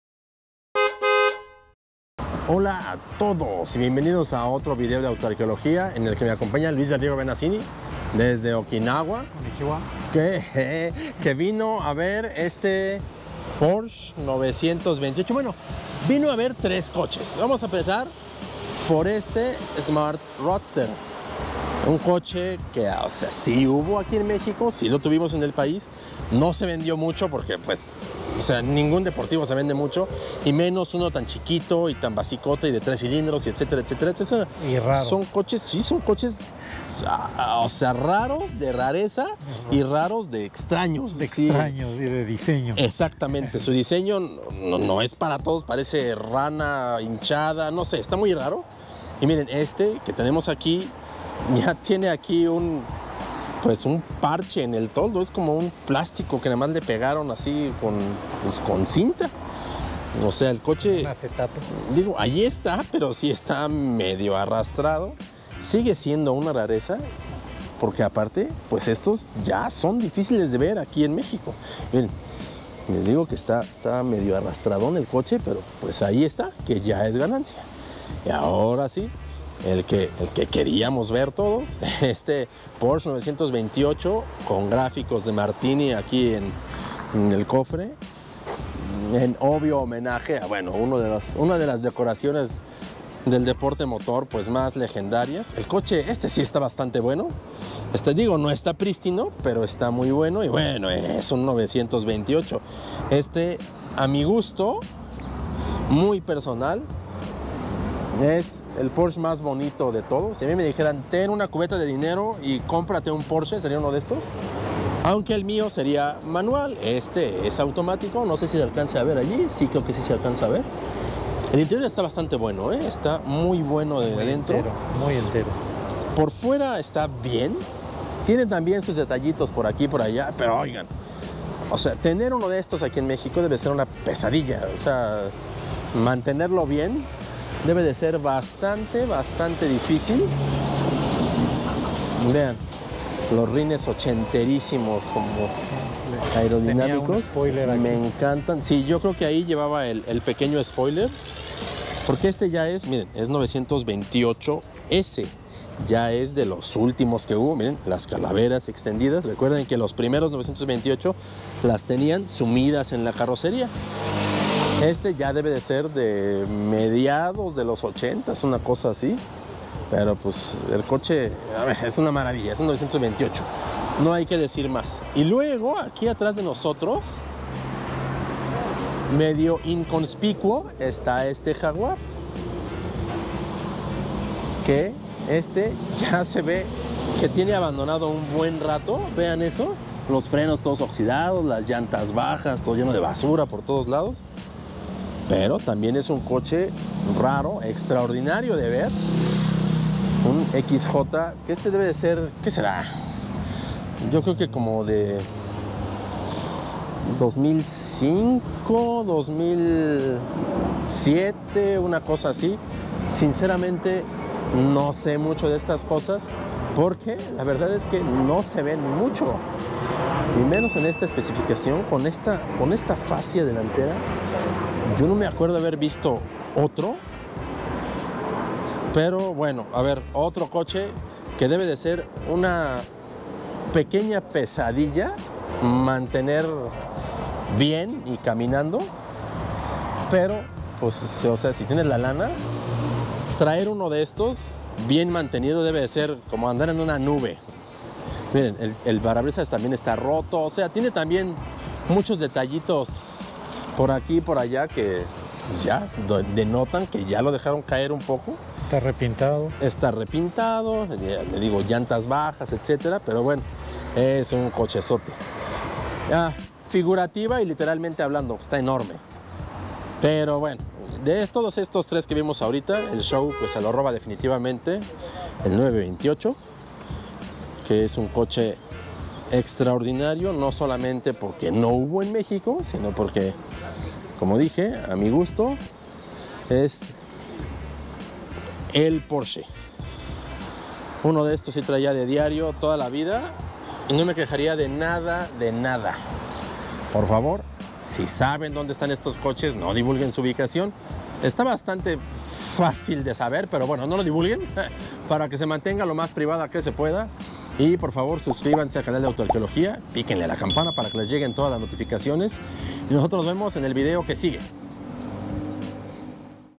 ¡Los 3 compadres europeos! Porsche, Jaaag & Smart / AutoArqueología
¡No todos los días te encuentras un Porsche 928, un Smart Roadster y un Jaguar XJ conviviendo en el mismo lugar! Un encuentro extraordinario 👌